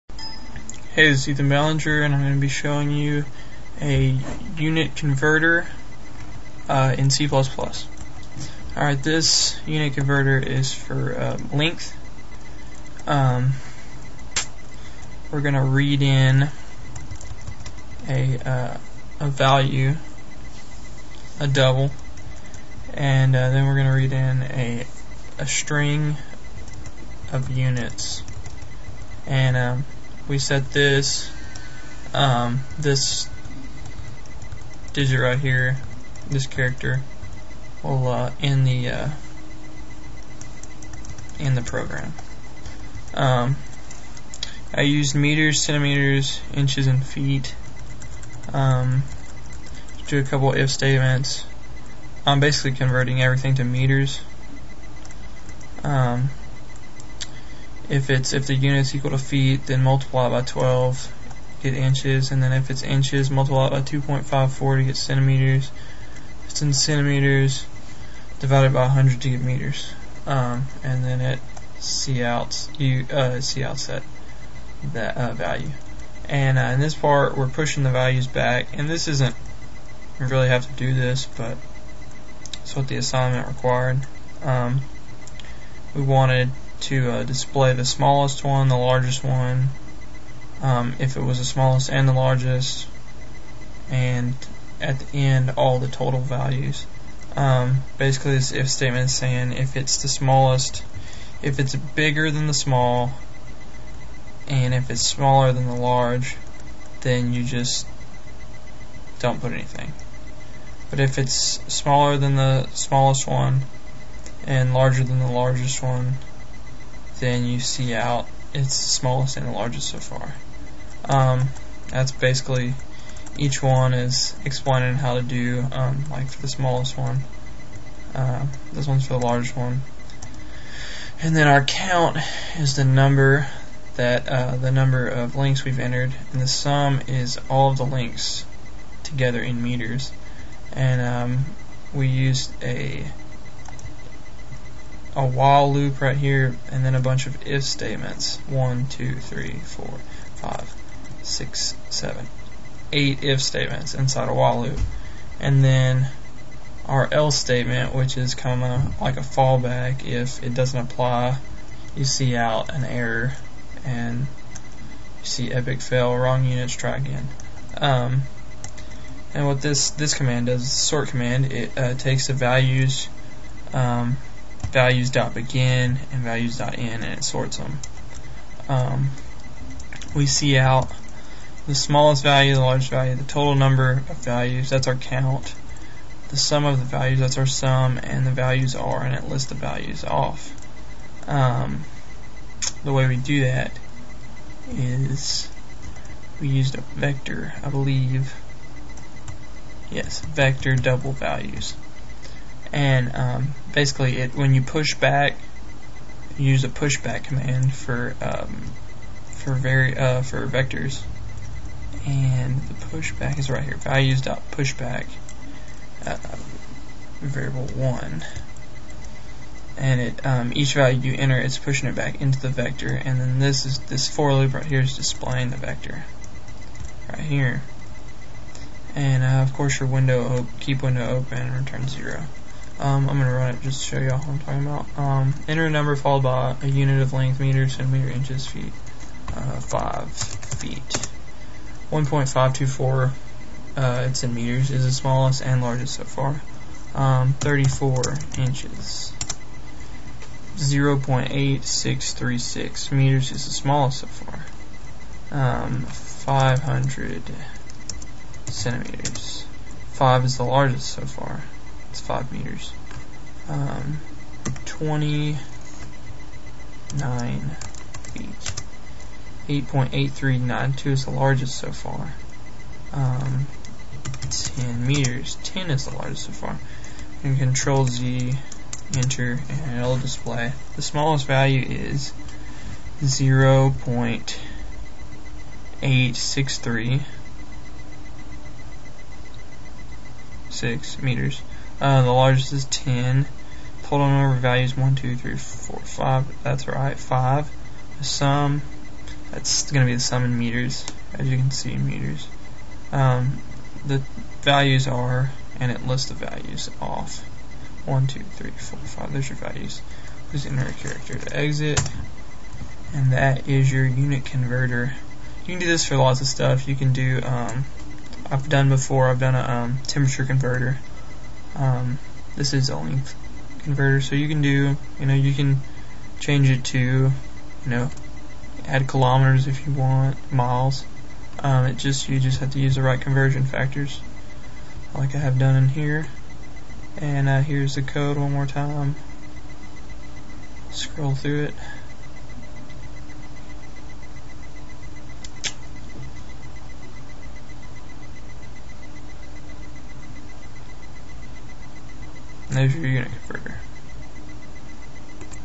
C++ Unit Converter
This video is a beginners tutorial to C++ using a basic length conversion program